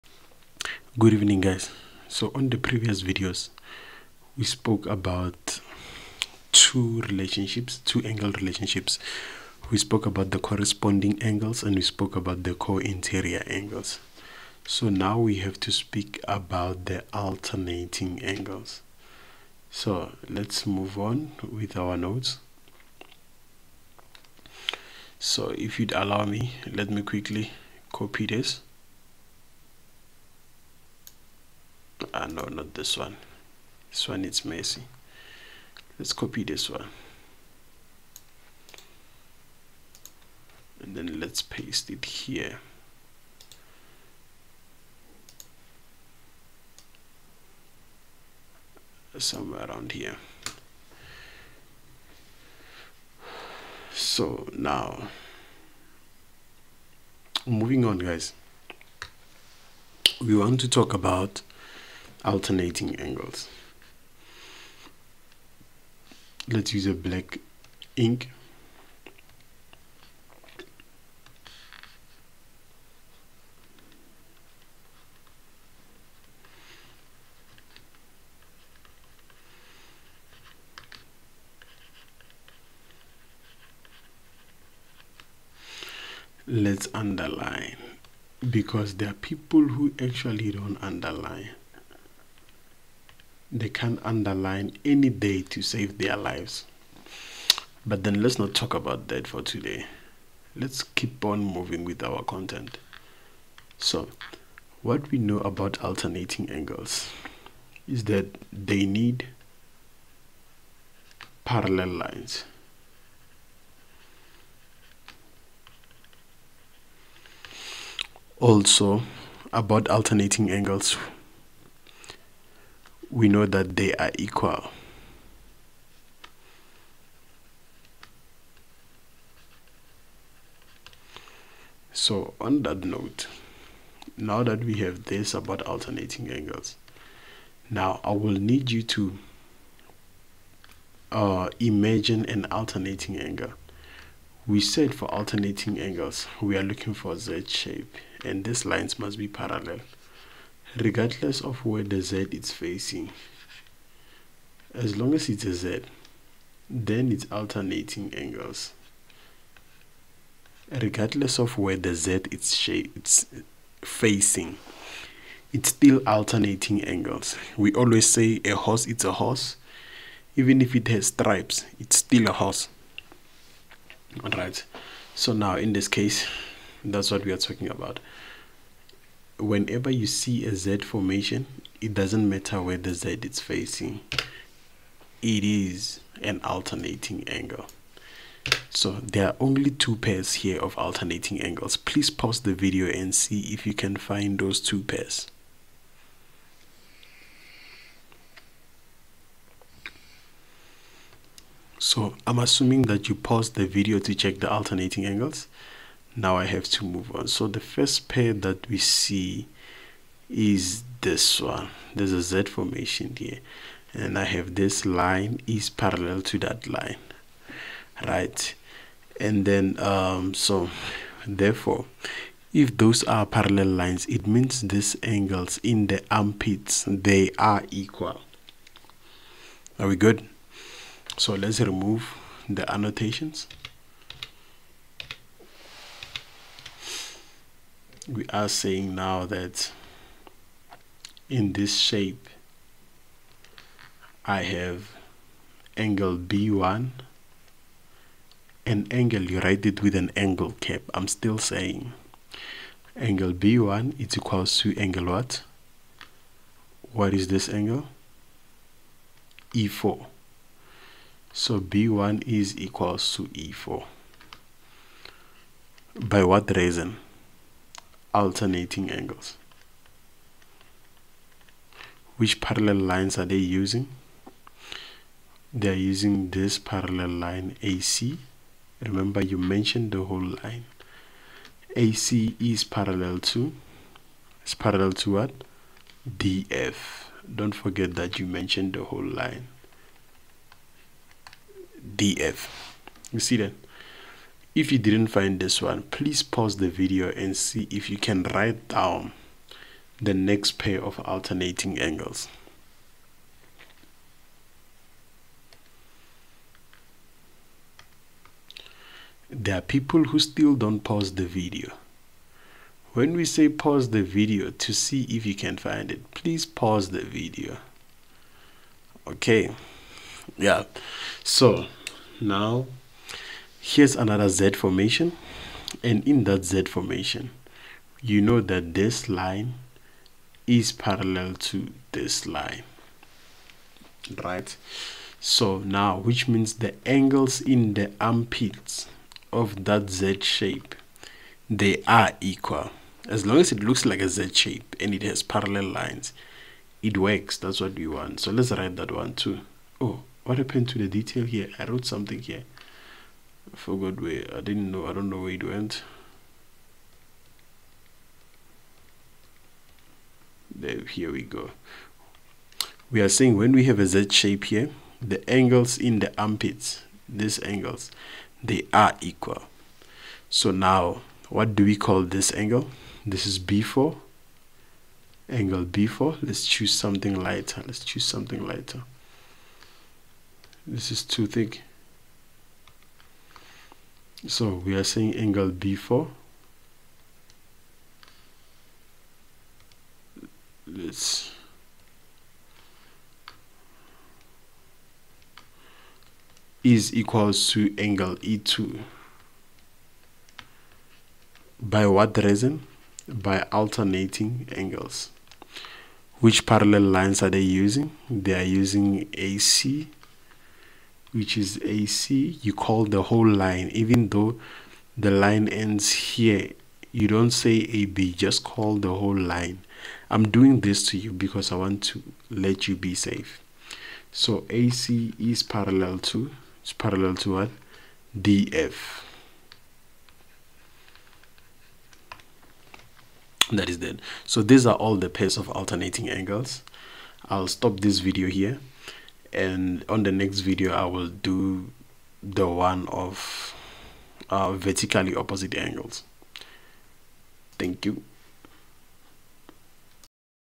Chapter: Geometry Of Straight Line: Angle Relationships| Alternating Angles
If you feel like the content is still too easy, do keep , moving, it gets better! This is still an introductory level.